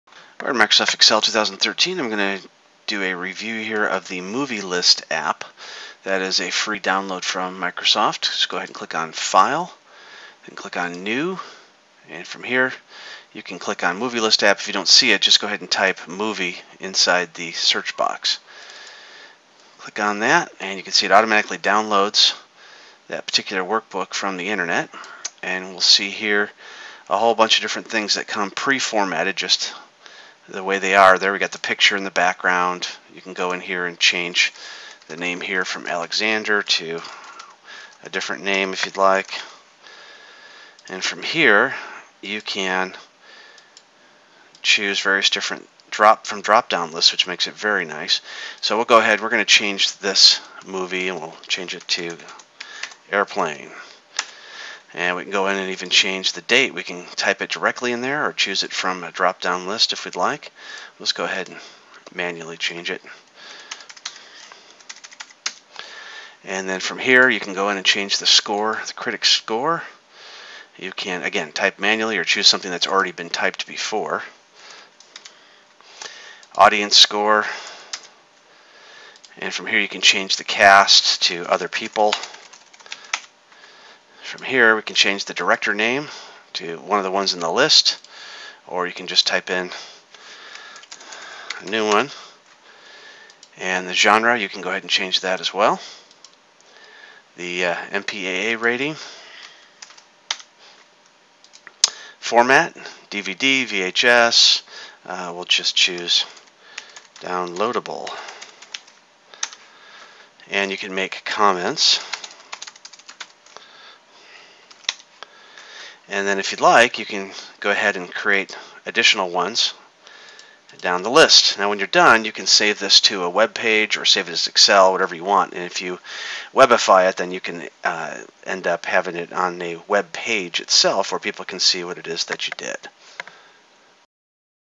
Review of the Movie App template in Microsoft Excel 2013
Author, teacher and talk show host Robert McMillen shows you review of the Movie app template in Microsoft Excel 2013